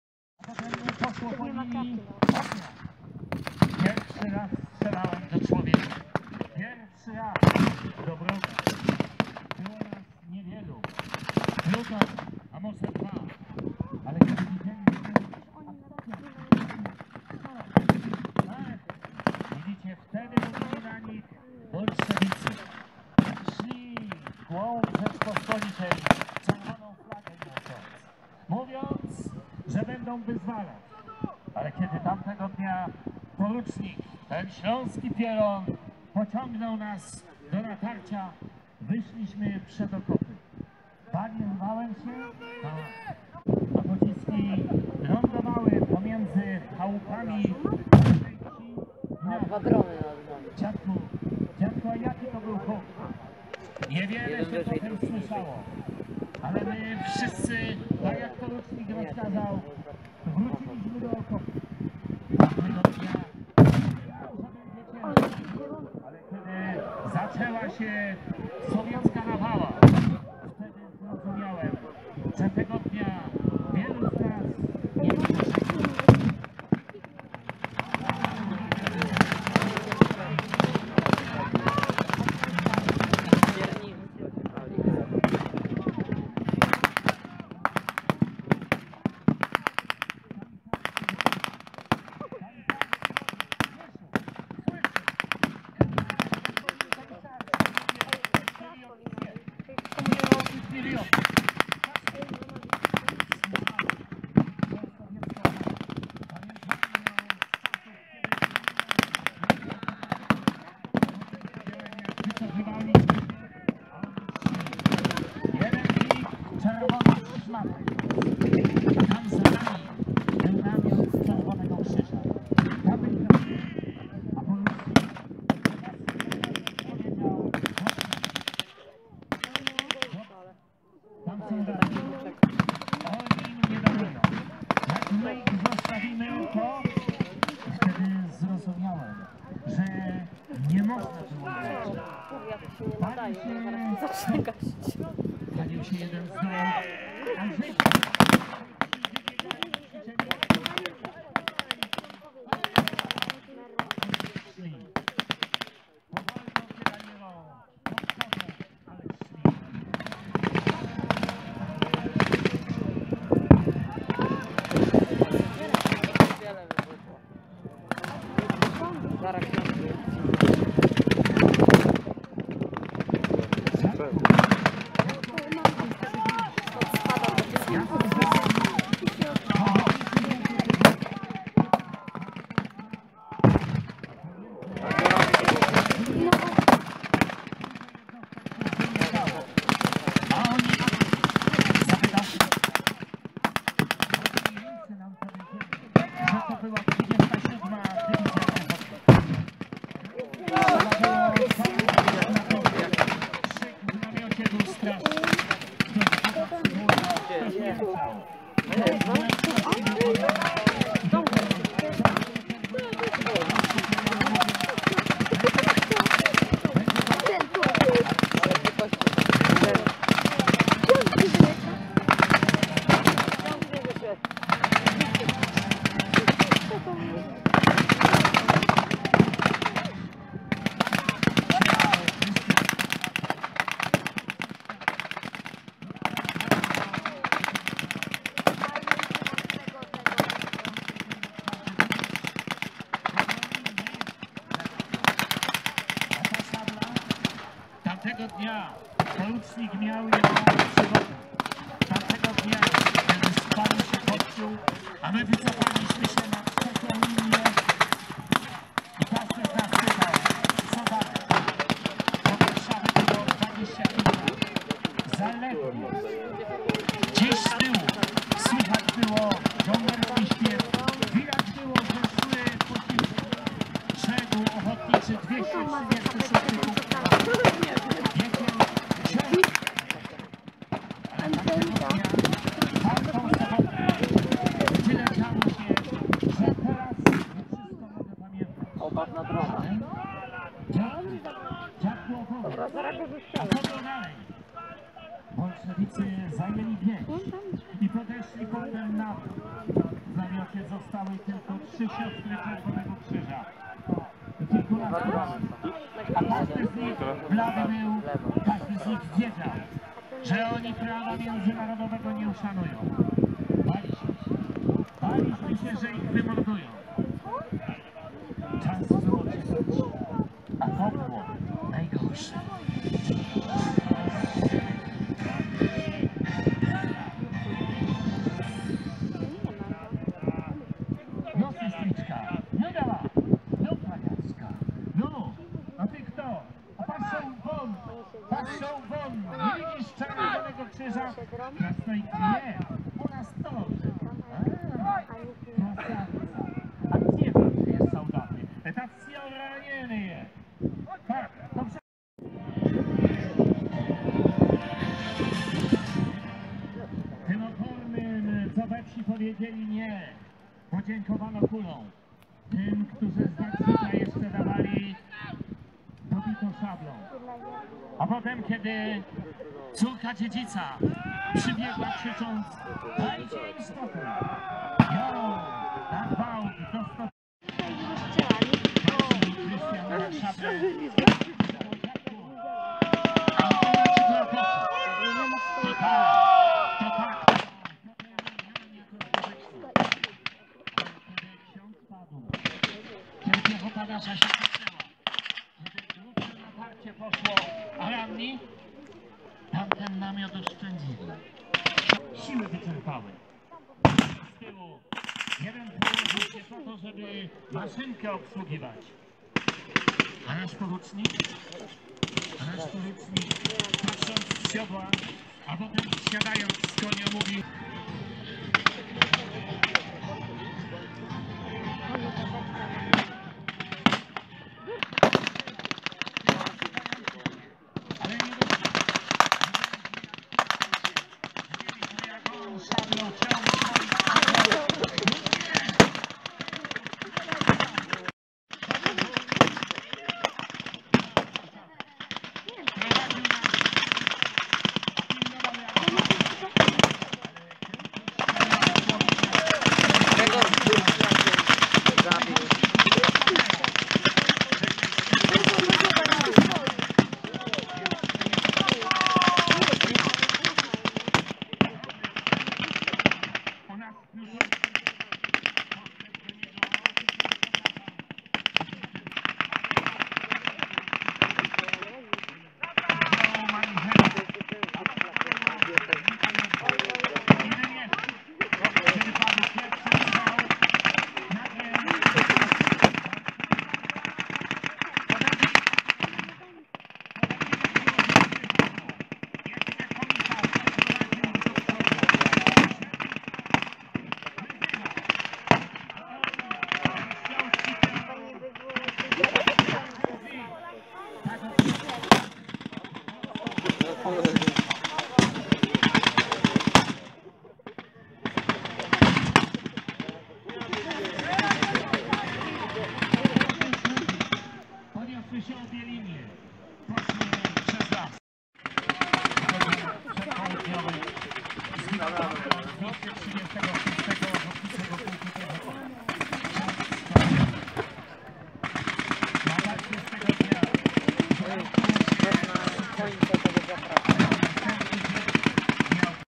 Opatów 04.09.2021 | A więc wojna! | rekonstrukcja historyczna | wojna polsko-bolszewicka | 1920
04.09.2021 w Opatowie odbyła się rekonstrukcja historyczna